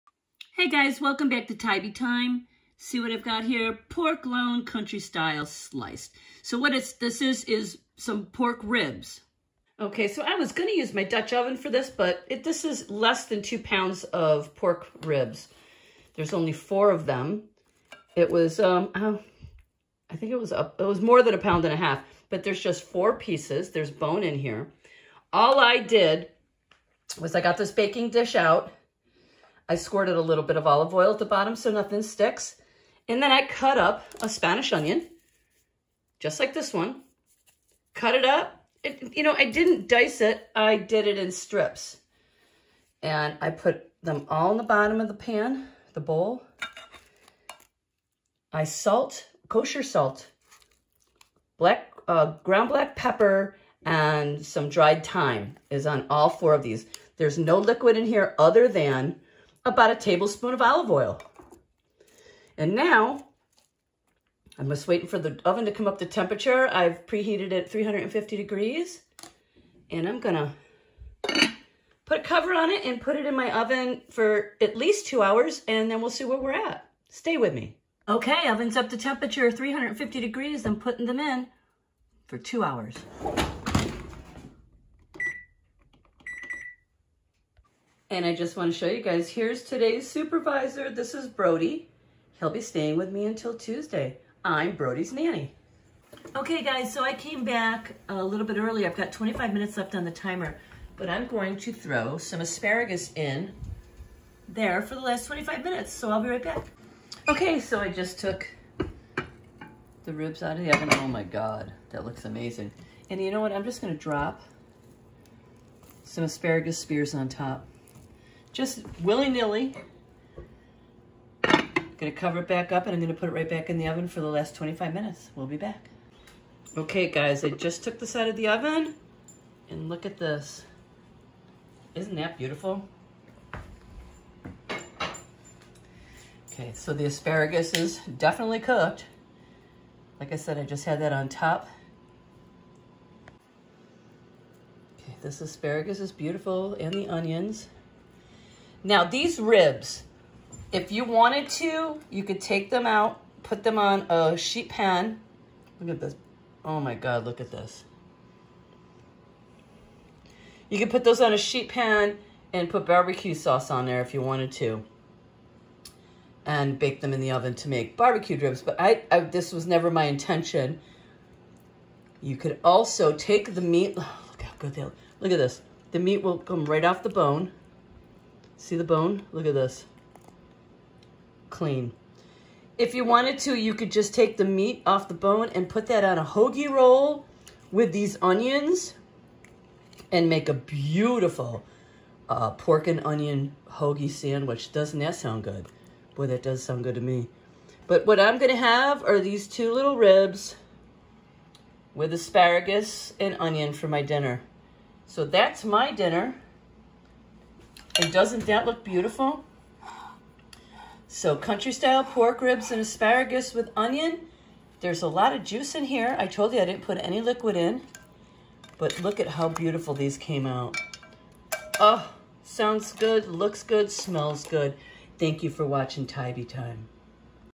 Country Style Pork Ribs Oven Baked
This is So Easy… You can adjust this recipe in many different ways. Add your favorite BBQ sauce at the end..Or,  Make A Tasty Hoagie .